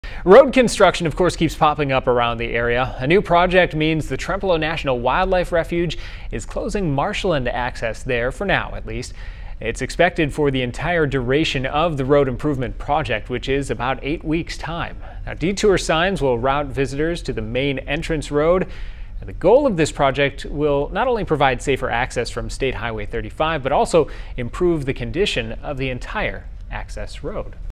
Road closure in the Trempealeau National Wildlife Refuge
A new project means the refuge is closing marshland access for now.